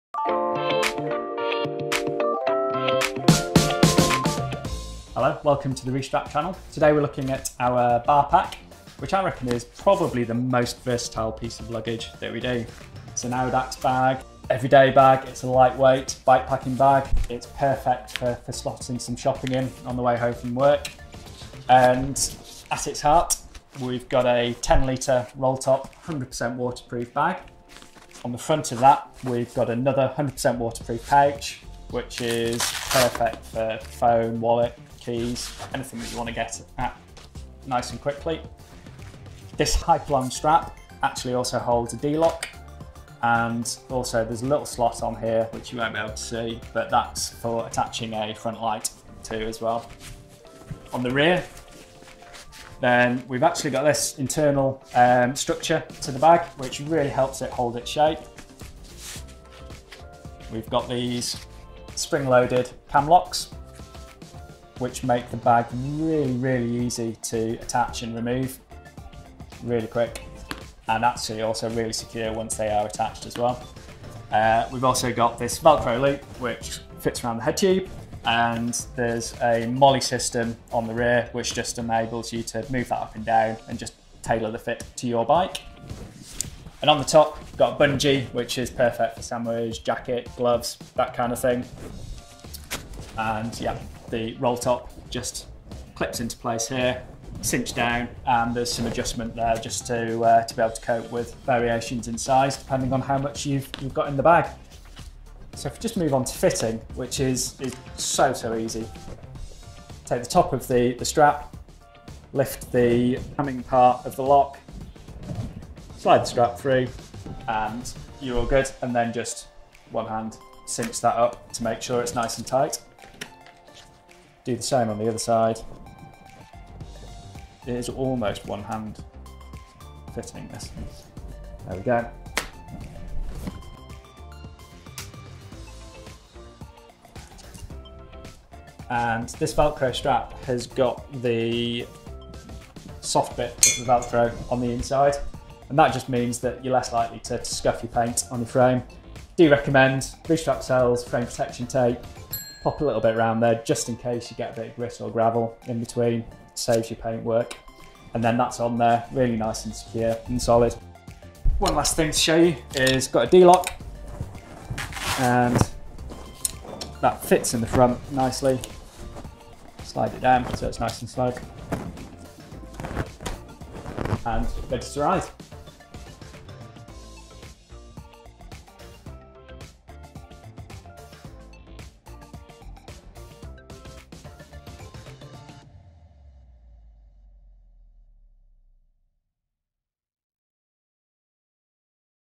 RESTRAP 大容量把手包 BAR PACK
Ideal for shop runs, off road adventures or audaxes, the Bar Pack is the perfect all day companion. Two fully waterproof roll top compartments provide 10 litres of capacity with a hypalon closure, securing the front compartment and providing a mounting slot for a clip-on front light or additional strapping. A mini D-lock holster sits between the two compartments, with the front compartment closure doubling as a securing strap for a lock for any urban excursions.

Structural rigidity is provided by an internal stiffener, a compact pack is guaranteed at all times due to the side-release tension and buckle system. An additional hard shell back provides further rigidity and a MOLLE adjustable headtube retention system guarantees a secure fit to your bike.

Foam spacers and spring-loaded cam locks provide a stable and reliable attachment to your handlebars an elasticated drawcord provides perfect on-the go storage for gloves, jackets and other accessories.

Handmade in our Yorkshire workshop from 100% waterproof textured nylon, with a nylon lining and finished with the classic vegan-friendly PU leather Restrap label.

Weight - 585g
Capacity - 10L